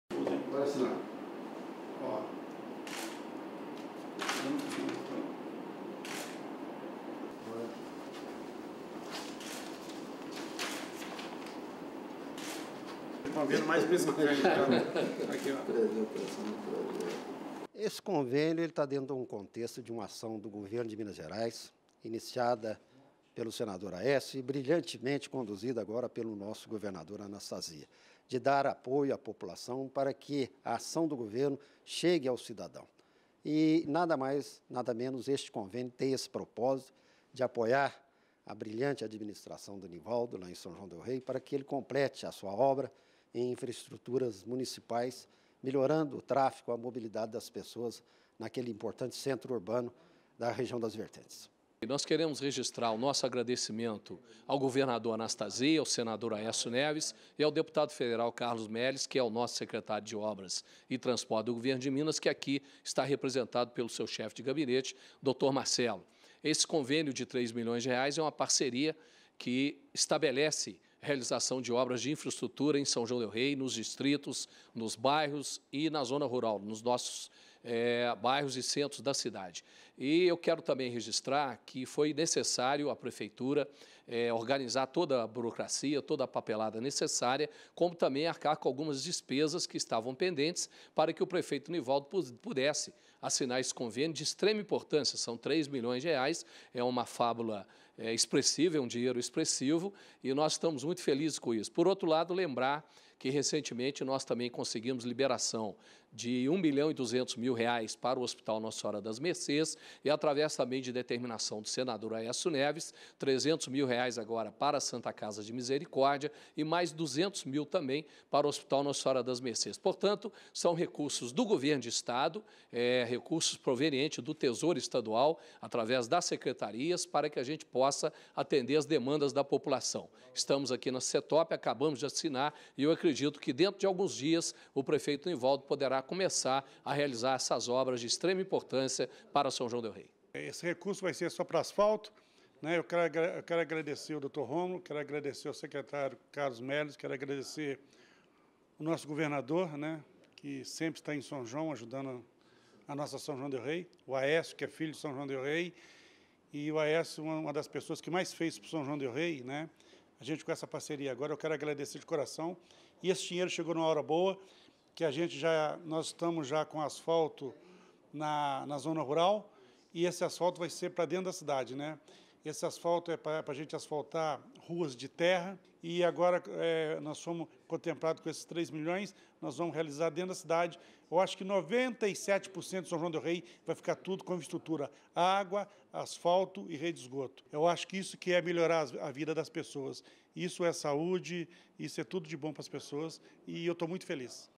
Convênio SETOP
Sonoras:
Marcelo Franco - Chefe de Gabinete do SETOP
Rômulo Viegas - Deputado Estadual
Nivaldo Andrade - Prefeito de São João Del Rei